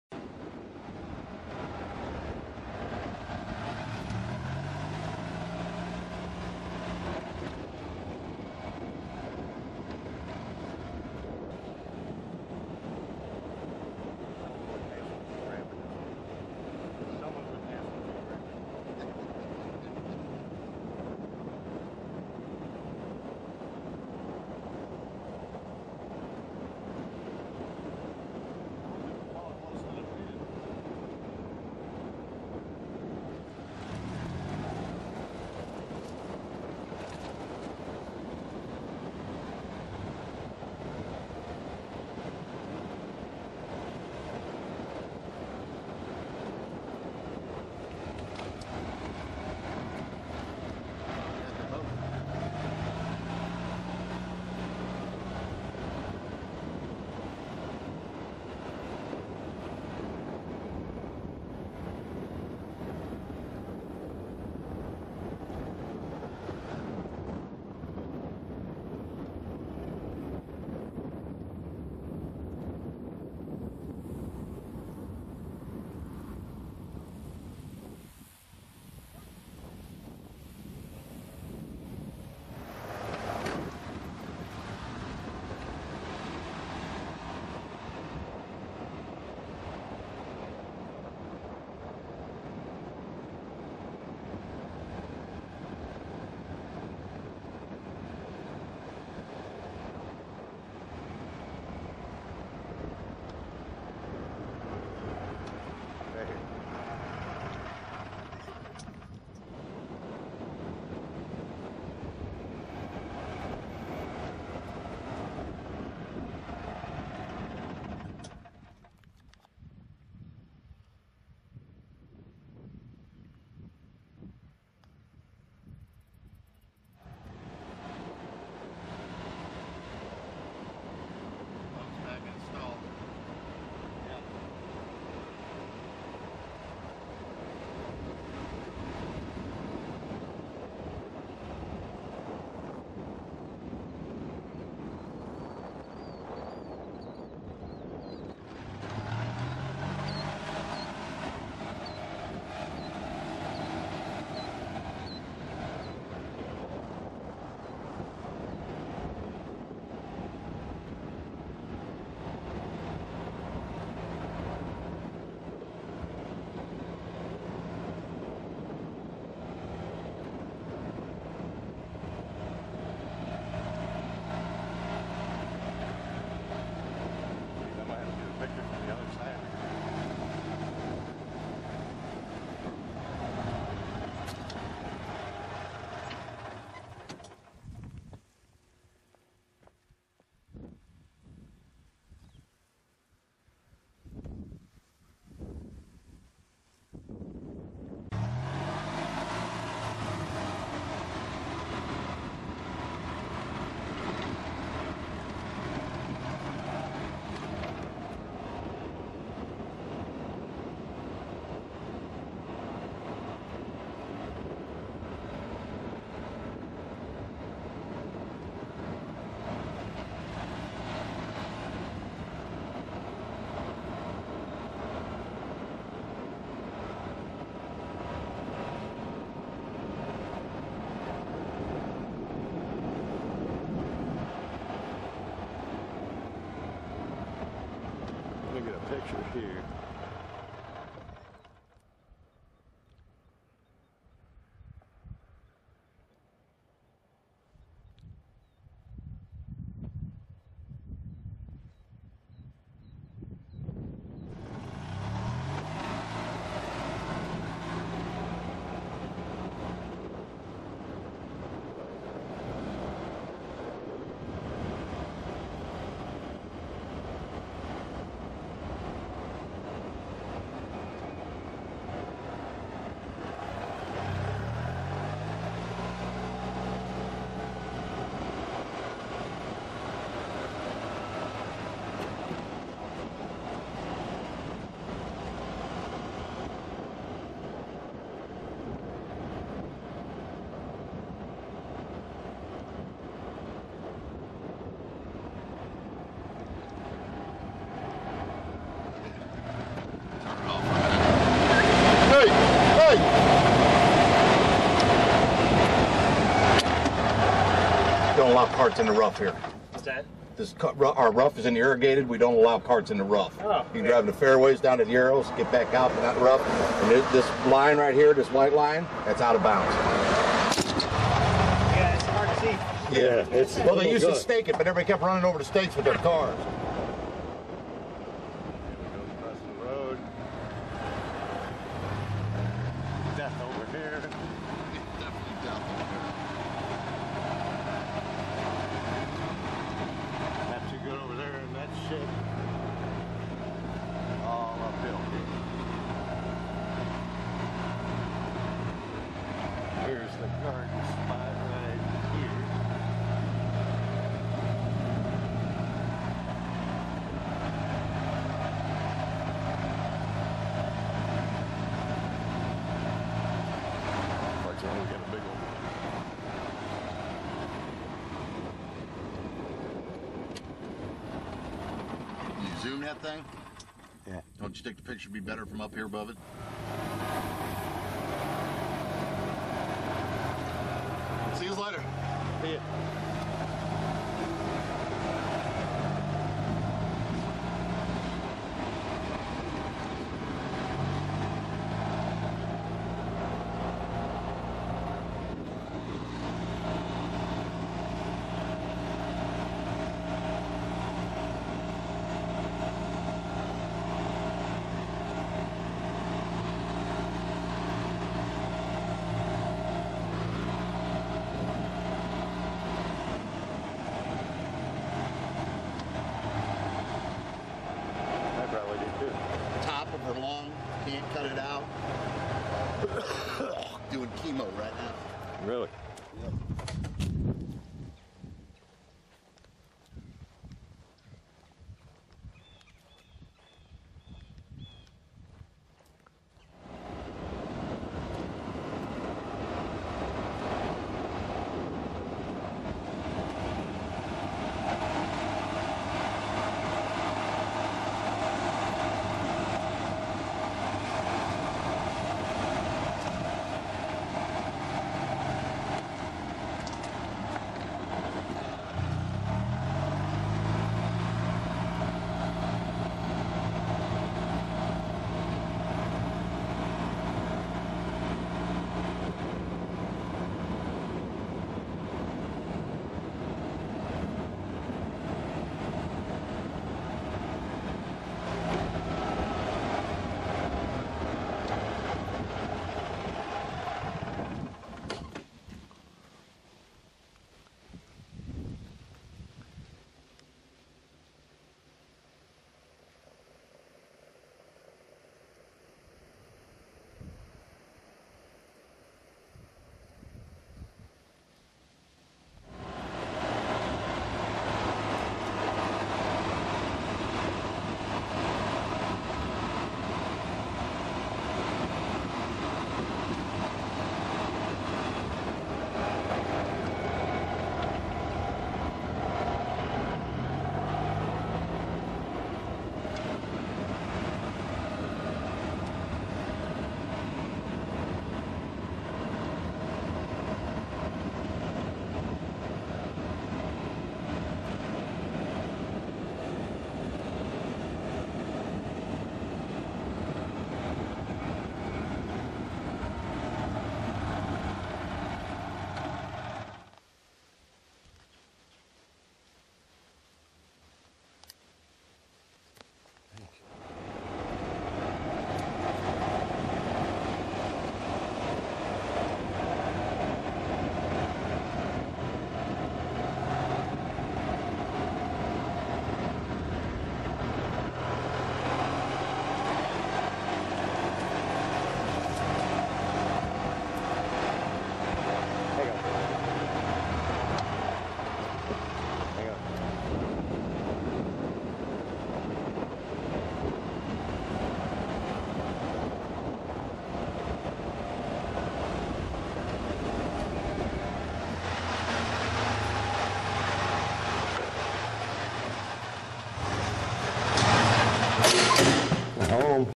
Incline village back nine
Back nine holes at the Golf Club at Incline Village.